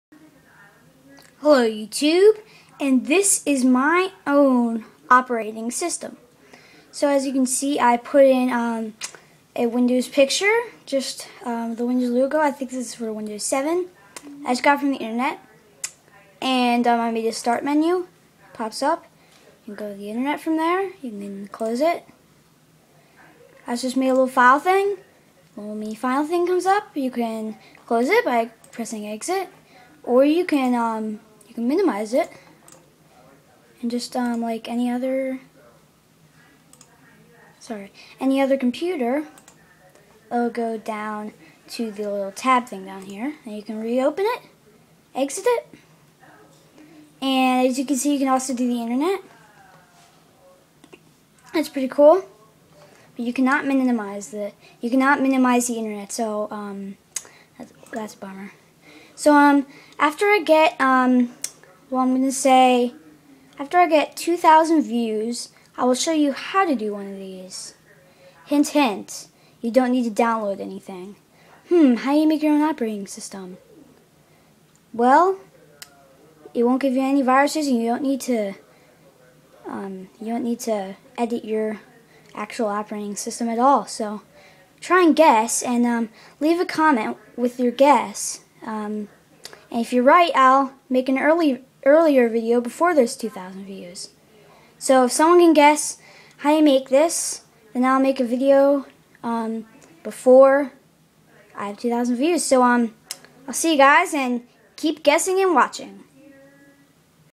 My Own Operating System
My Own Operating System! After 2000 views I will post a video that shows how to do it. Or, if you guess how to do it and get it right, I will post the video before 2000 views.